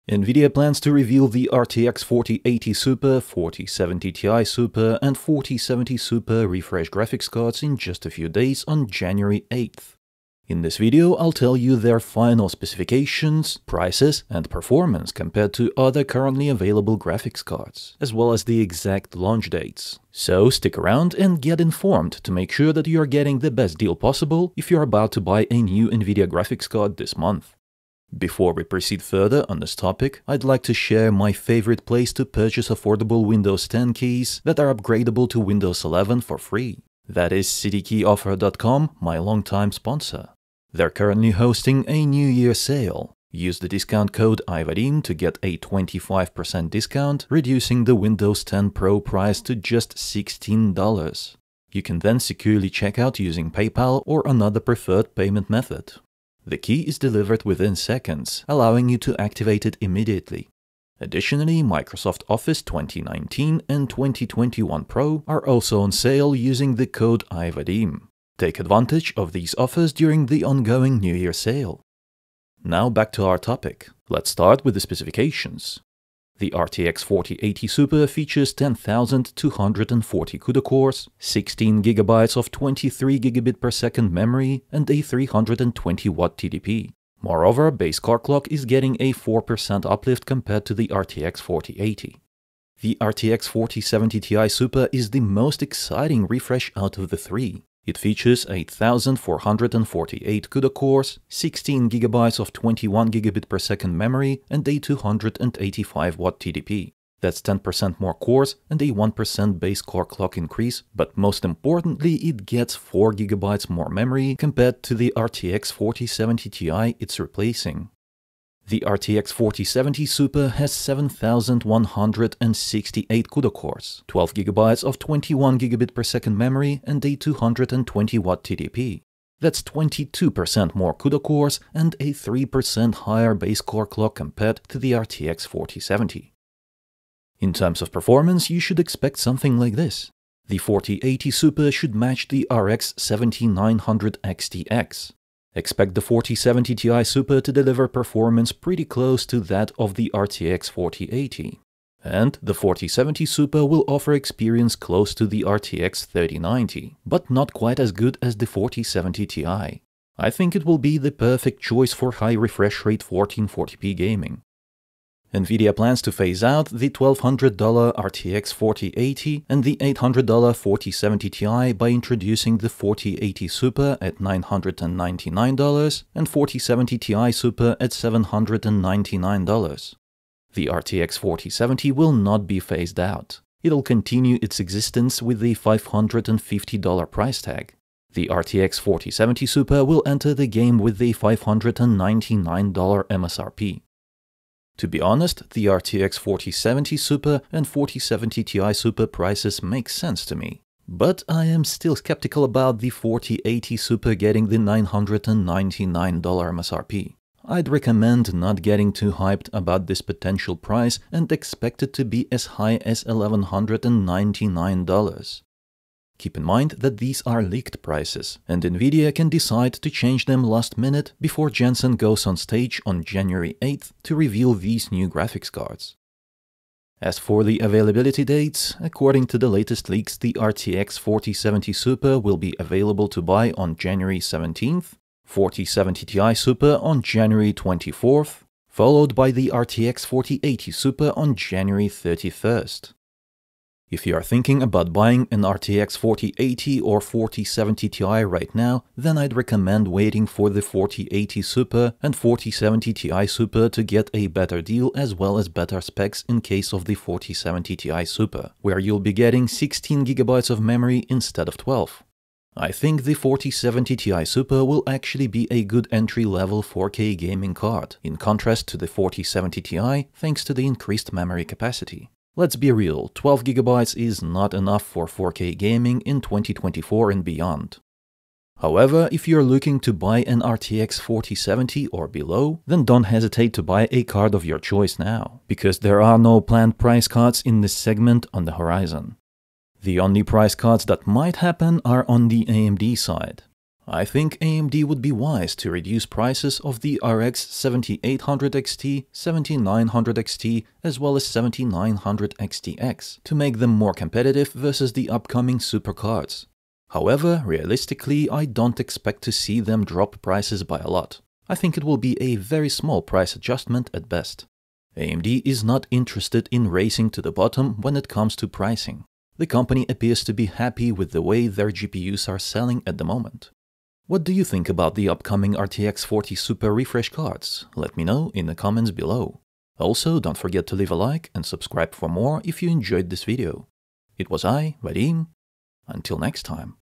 RTX 4080 SUPER, 4070 Ti SUPER, 4070 SUPER: Final Specs, Performance, Prices, Release Dates
In this video, I’ll tell you the RTX 4080 SUPER, RTX 4070 Ti SUPER and RTX 4070 SUPER graphics cards final specifications, prices and performance.

RTX 4090
RTX 4080 SUPER - from January 31st
RTX 4080
RTX 4070 Ti SUPER - from January 24th
RTX 4070 Ti
RX 7900 XTX
RX 7900 XT
RTX 4070 SUPER - from January 17th
RTX 4070
RX 7800 XT
RTX 4060 Ti 16GB
RTX 4060 Ti 8GB
RX 7700 XT
RX 7600 XT 16GB - from January 24th
RTX 4060
RTX 3060 12GB
RX 7600
RX 6600
RTX 3050

Timestamps:
0:00 - Intro
0:27 - Sponsor
1:14 - Specifications
2:17 - Performance
2:45 - Prices
3:46 - Launch dates
4:03 - Conslusion